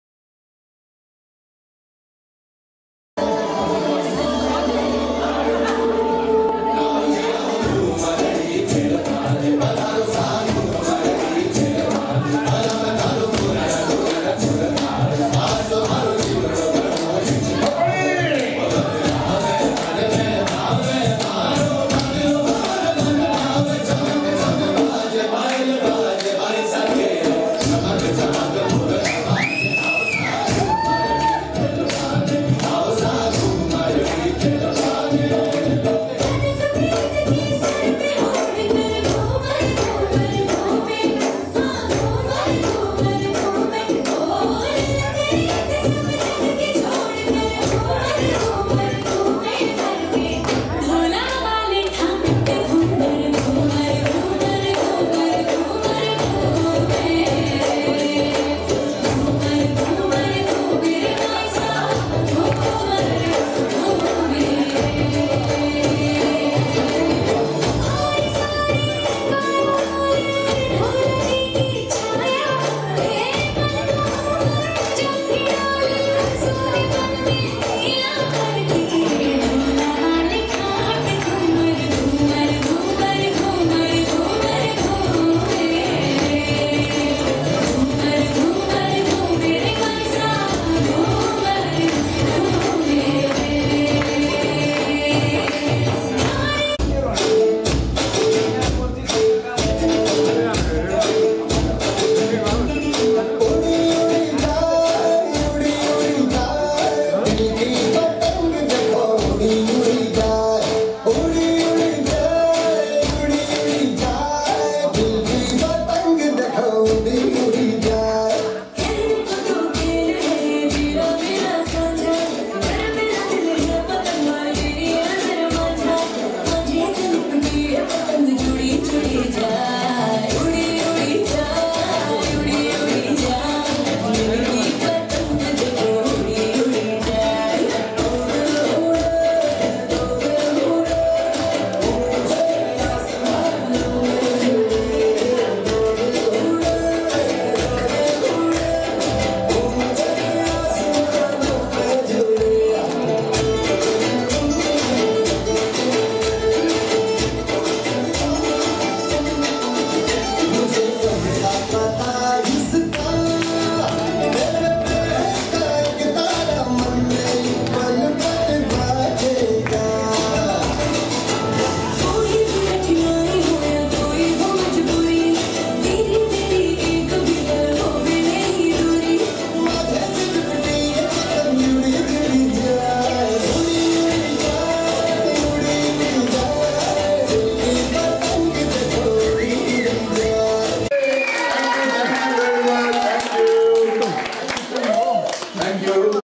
Part 06: Thanti Nepalese Society UK's New Year 2075 celebration (HD)
Thanti Nepalese Society UK organised a program in Harrow London on 14 April 2018, celebrating 2075 Nepali New Year .